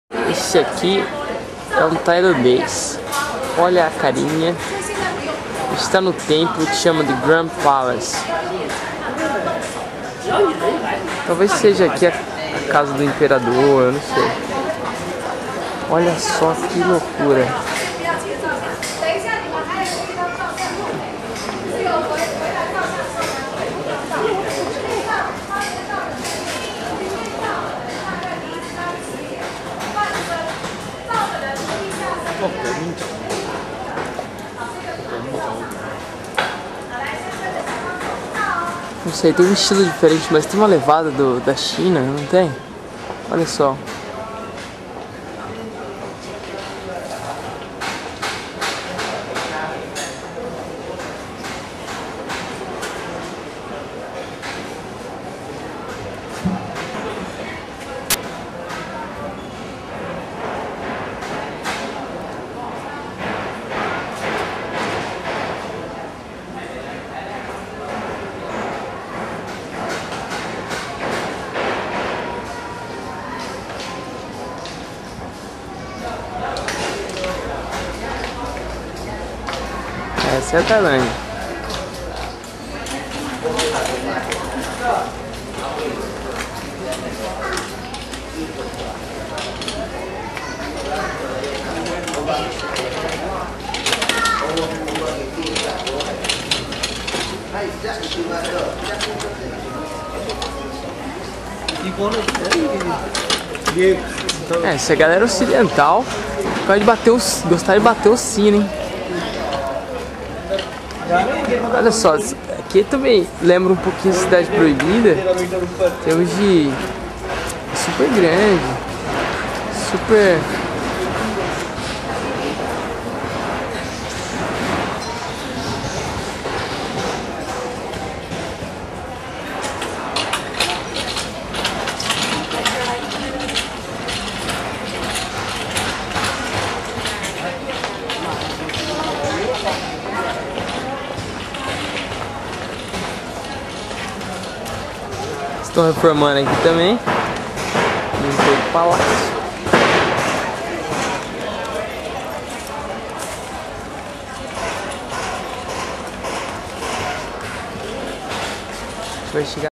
The Grand Palace 01of05
The Grand Palace (Thai: พระบรมมหาราชวัง, Phra Borom Maha Ratcha Wang) is a complex of buildings in Bangkok, Thailand. It served as the official residence of the king of Thailand from the 18th century to the mid-20th century.